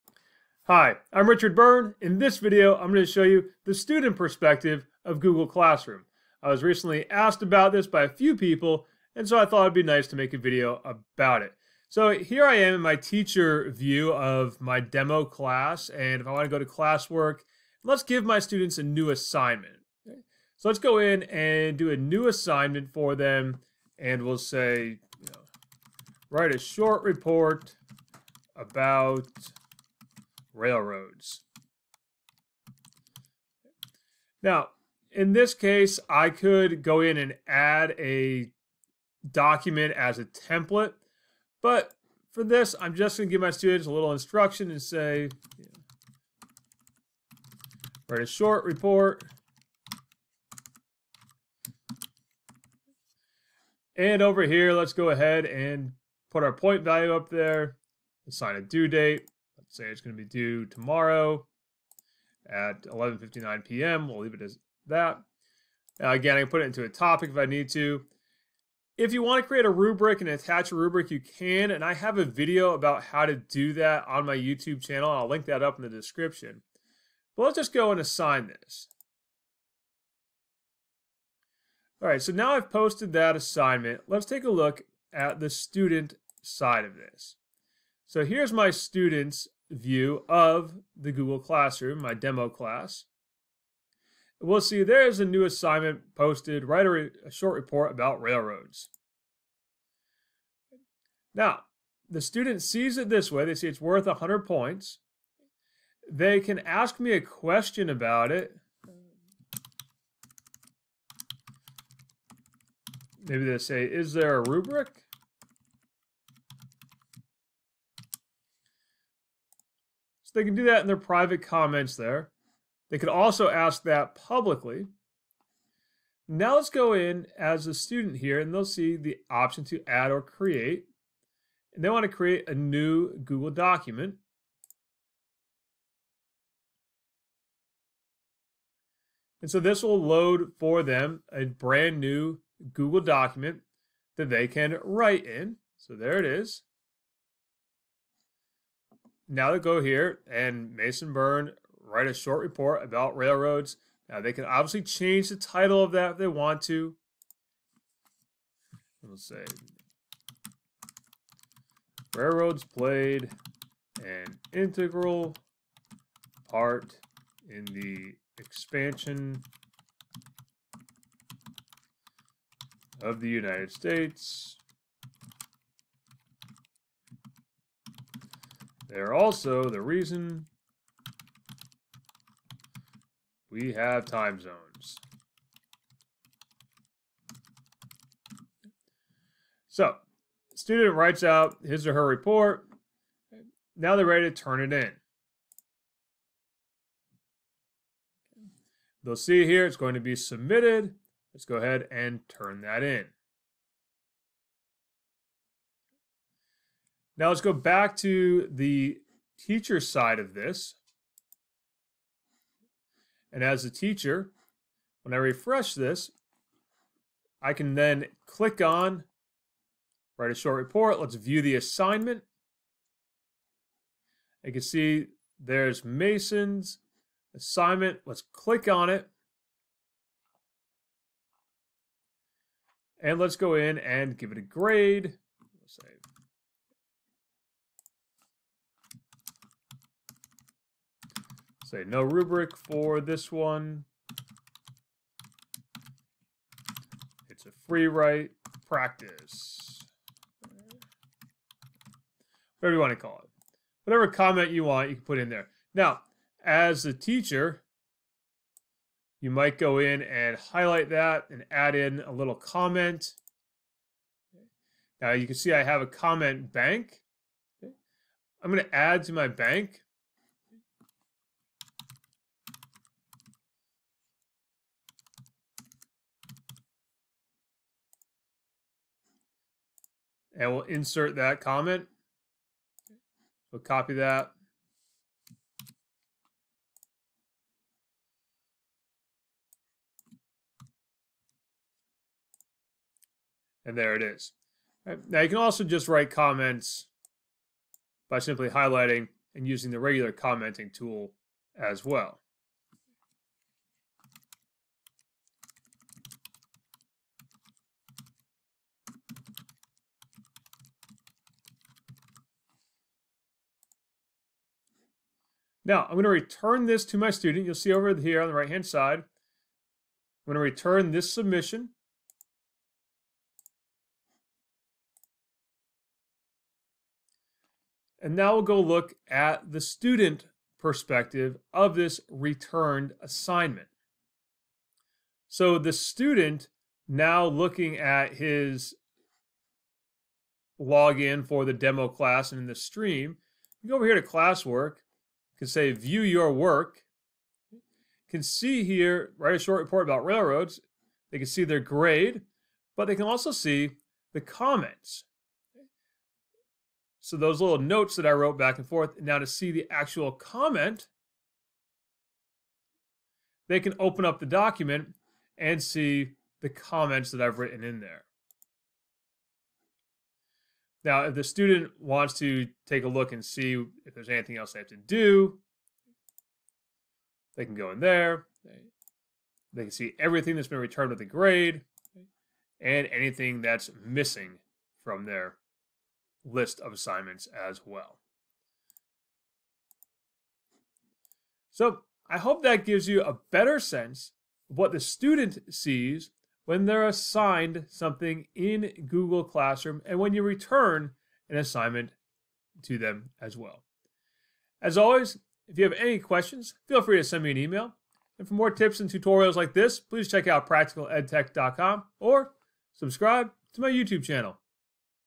Student View of Google Classroom Assignments
Watch this video if you have wondered what a students see when you give them assignments in Google Classroom or what they see when you return work to them in Google Classroom.